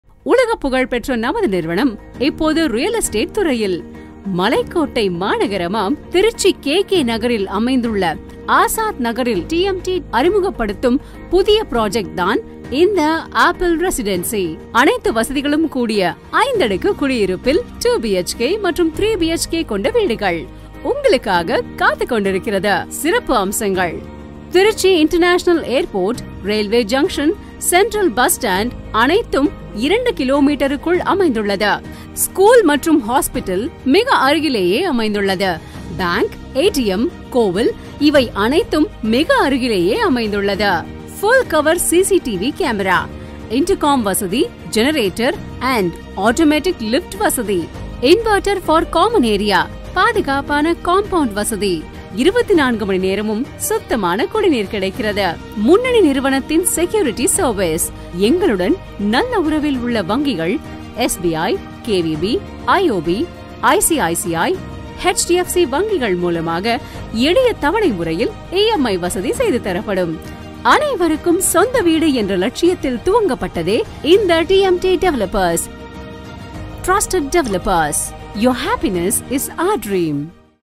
APPLE Residency@KK nagar,Trichy.[TMT developers]
"Five storied fabulous Apartment strategically located in KK Nagar, Trichy. in such a way that everything becomes easily accessible. 3 BHK and 2 BHK Available. Each floor containing 7 Houses. Total 35 Houses."